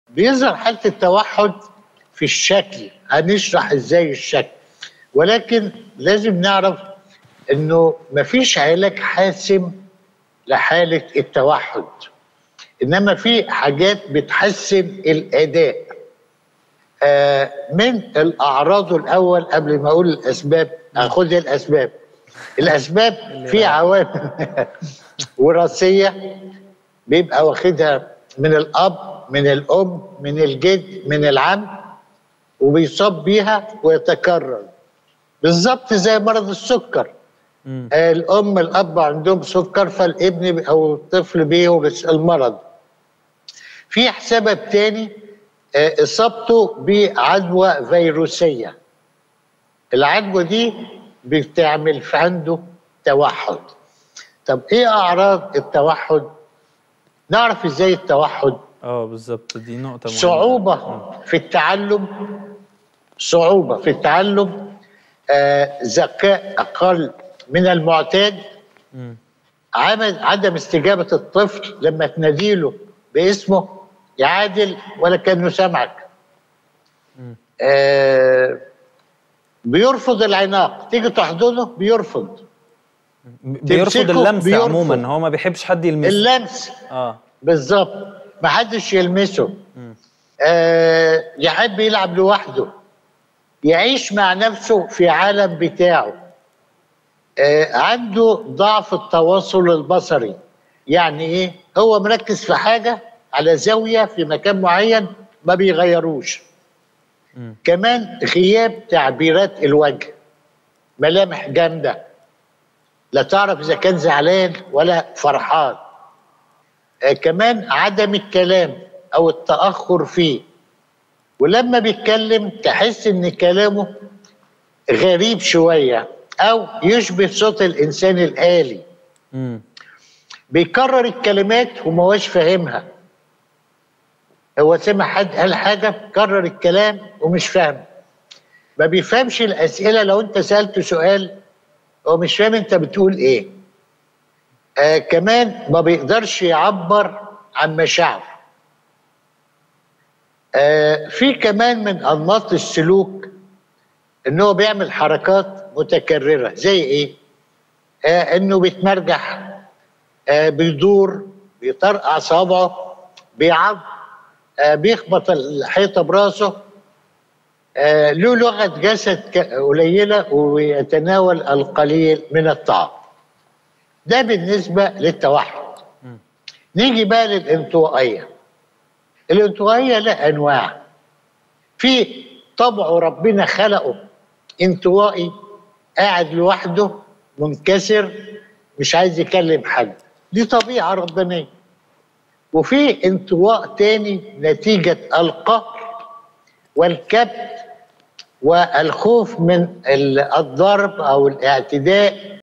ما أعراض التوحد؟ د. علاء الغندور يجيب.
ما أعراض التوحد؟
د. علاء الغندور يجيب.